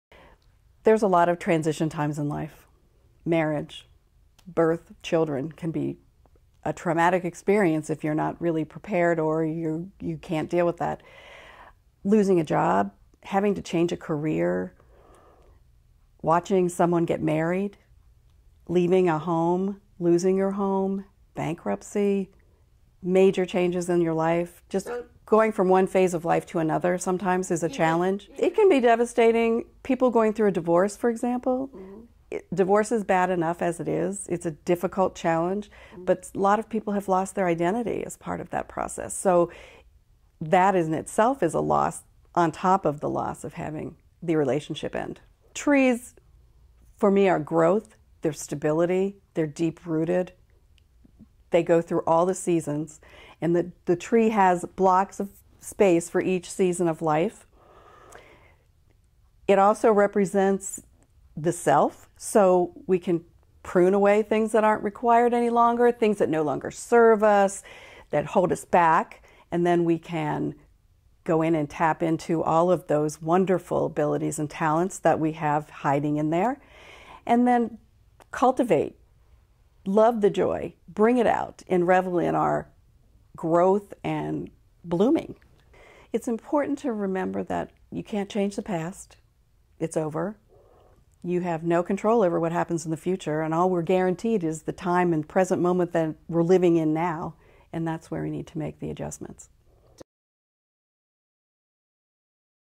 Life Transitions with Monica Vance
Life Transitions take many forms. Monica Vance Professional Hypnosis eases the difficulties encountered.